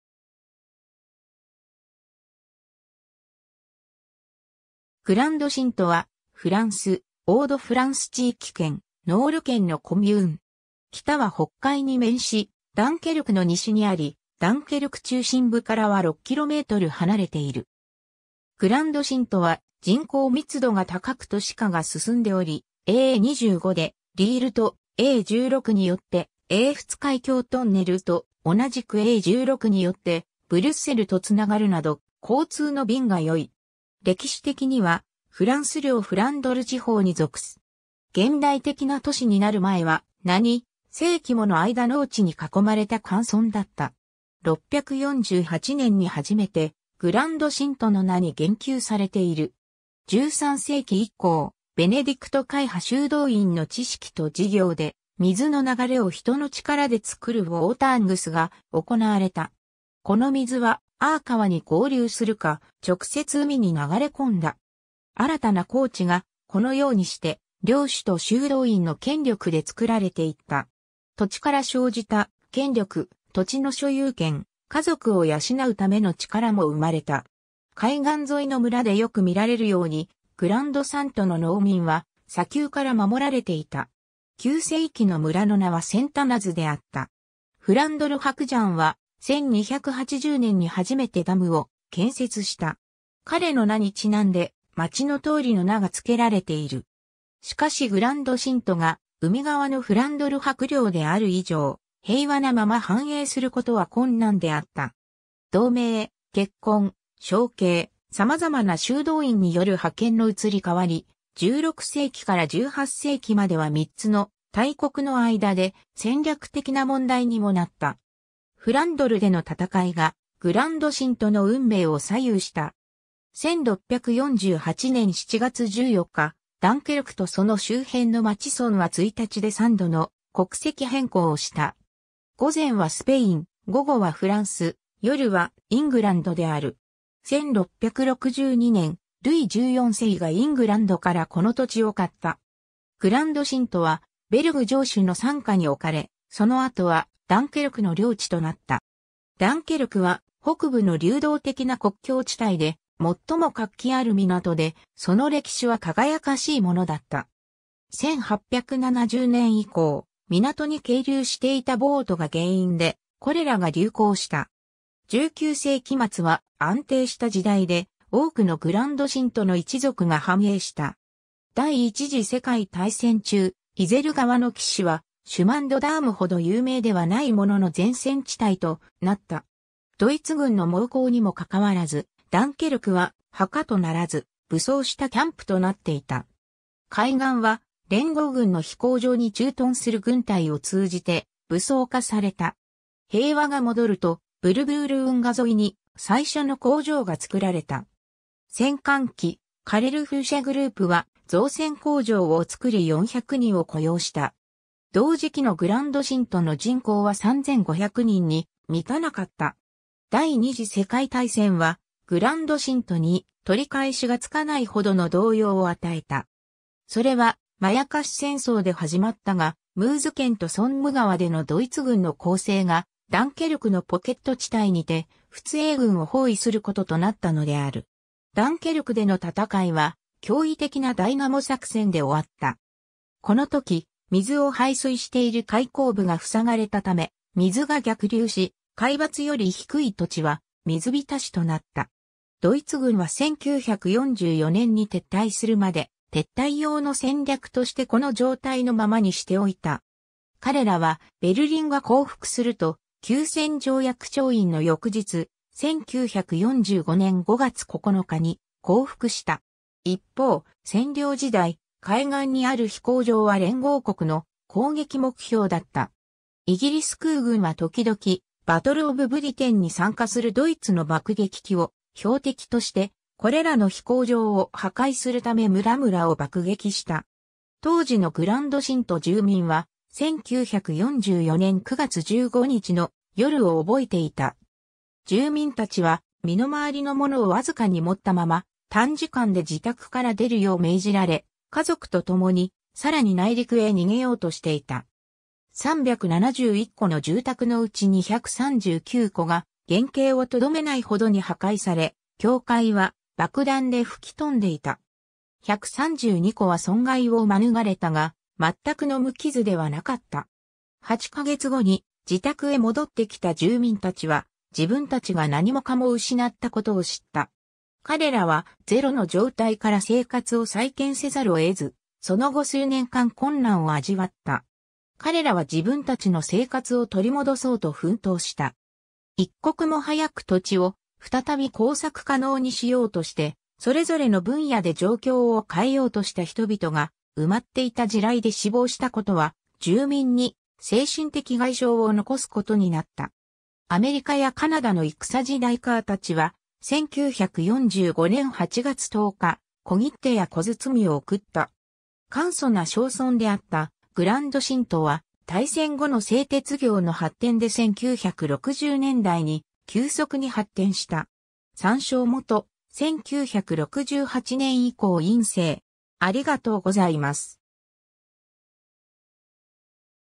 グランド＝シント
グランド＝シント （Grande-Synthe、オランダ語:Groot-Sinten）は、フランス、オー＝ド＝フランス地域圏、ノール県のコミューン。
 北は北海に面し、ダンケルクの西にあり、ダンケルク中心部からは6km離れている。
グランド＝シントは人口密度が高く都市化が進んでおり、A25でリールと、A16によって英仏海峡トンネルと、同じくA16によってブリュッセルとつながるなど、交通の便がよい。
 歴史的には、フランス領フランドル地方に属す。
 現代的な都市になる前は、何世紀もの間農地に囲まれた寒村だった。
648年に初めてグランド＝シントの名に言及されている。
13世紀以降、ベネディクト会派修道院の知識と事業で、水の流れを人の力でつくるwateringuesが行われた。
この水はアー川に合流するか、直接海に流れ込んだ。
新たな耕地が、このようにして領主と修道院の権力で作られていった。
土地から生じた権力、土地の所有権、家族を養うための力も生まれた。
海岸沿いの村でよく見られるように、グランド＝サントの農民は砂丘から守られていた。
9世紀の村の名はSentinasであった。
フランドル伯ジャンは、1280年に初めてダムを建設した。
彼の名にちなんでまちの通りの名がつけられている。
しかしグランド＝シントが海側のフランドル伯領である以上、平和なまま繁栄することは困難であった。
同盟、結婚、承継、さまざまな修道院による覇権の移り変わり、16世紀から18世紀までは3つの大国（低地諸国を治めることになるスペイン、イングランド、フランス）の間で戦略的な問題にもなった。
フランドルでの戦いがグランド＝シントの運命を左右した。
1648年7月14日、ダンケルクとその周辺の町村は1日で3度の国籍変更をした。
午前はスペイン、午後はフランス、夜はイングランドである。
1662年、ルイ14世がイングランドからこの土地を買った。
グランド＝シントはベルグ城主の傘下におかれ、その後はダンケルクの領地となった。
ダンケルクは、北部の流動的な国境地帯で最も活気ある港で、その歴史は輝かしいものだった。
1870年以降、港に係留していたボートが原因でコレラが流行した。
19世紀末は安定した時代で、多くのグランド＝シントの一族が繁栄した。
 第一次世界大戦中、イゼル川の岸は、シュマン・ド・ダーム（fr）ほど有名ではないものの前線地帯となった。
ドイツ軍の猛攻にもかかわらず、ダンケルクは墓とならず、武装したキャンプとなっていた。
海岸は、連合軍の飛行場に駐屯する軍隊を通じて武装化された。
平和が戻ると、ブルブール運河沿いに最初の工場がつくられた。
戦間期、カレル・フーシェ・グループは造船工場をつくり400人を雇用した。
同時期のグランド＝シントの人口は3500人に満たなかった。
 第二次世界大戦は、グランド＝シントに取り返しがつかないほどの動揺を与えた。
それはまやかし戦争で始まったが、ムーズ県とソンム川でのドイツ軍の攻勢が、ダンケルクのポケット地帯にて仏英軍を包囲することとなったのである。
ダンケルクでの戦いは、驚異的なダイナモ作戦で終わった。
この時、水を排水している開口部が塞がれたため水が逆流し、海抜より低い土地は水浸しとなった。
ドイツ軍は1944年に撤退するまで、撤退用の戦略としてこの状態のままにしておいた。
彼らはベルリンが降伏すると、休戦条約調印の翌日、1945年5月9日に降伏した。
一方、占領時代、海岸にある飛行場は連合国の攻撃目標だった。
イギリス空軍は時々、バトル・オブ・ブリテンに参加するドイツの爆撃機を標的として、これらの飛行場を破壊するため村々を爆撃した。
当時のグランド＝シント住民は、1944年9月15日の夜を覚えていた。
住民たちは身の回りの物をわずかに持ったまま、短時間で自宅から出るよう命じられ、家族とともにさらに内陸へ逃げようとしていた。
371戸の住宅のうち239戸が原型をとどめないほどに破壊され、教会は爆弾で吹き飛んでいた。
132戸は損害を免れたが全くの無傷ではなかった。
 8ヵ月後に自宅へ戻ってきた住民たちは、自分たちが何もかも失ったことを知った。
彼らはゼロの状態から生活を再建せざるを得ず、その後数年間困難を味わった。
彼らは自分たちの生活を取り戻そうと奮闘した。
一刻も早く土地を再び耕作可能にしようとして、それぞれの分野で状況を変えようとした人々が埋まっていた地雷で死亡したことは、住民に精神的外傷を残すことになっ...